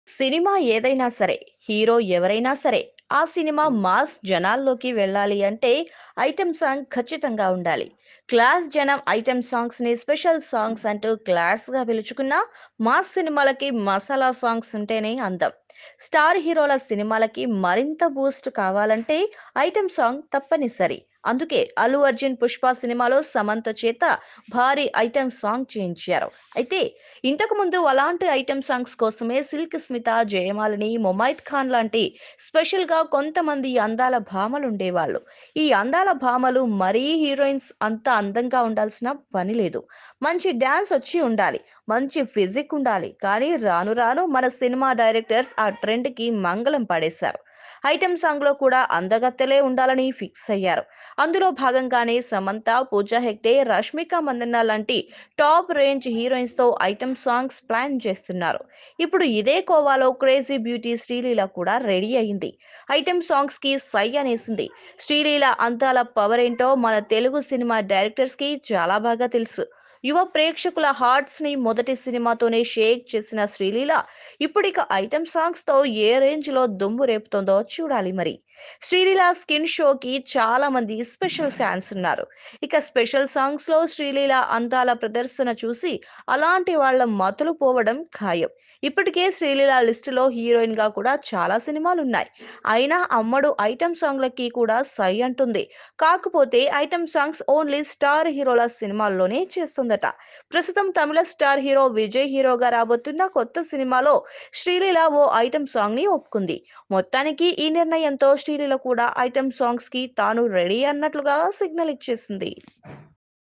ఐటమ్ సాంగ్స్ కి సై అంటున్నా క్రేజీ బ్యూటీ || Star Heroin Green signal To Item Songs || Sreeleela
Watch Star Heroin Green signal To Item Songs Video In Oktelugu Entertainment .